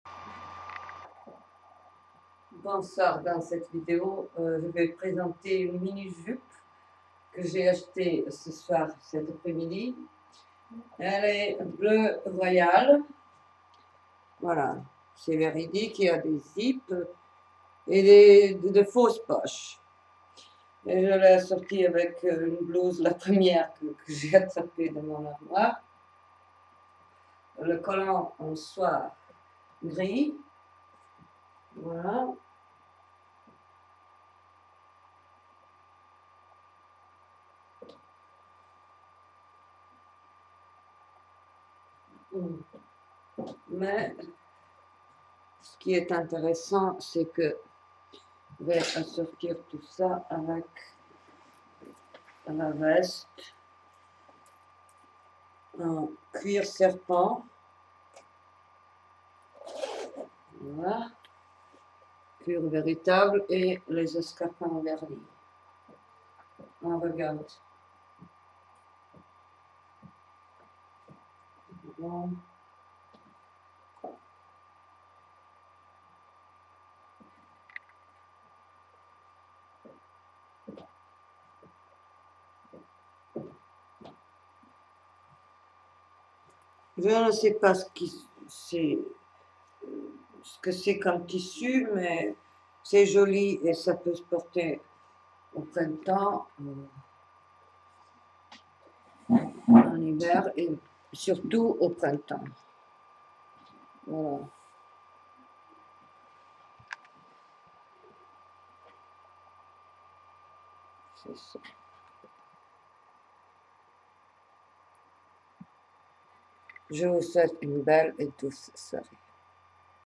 mini jupe bleu royal
jupe courte avec les zip sur les fausses poches laterales et veste en cuir serpent veritable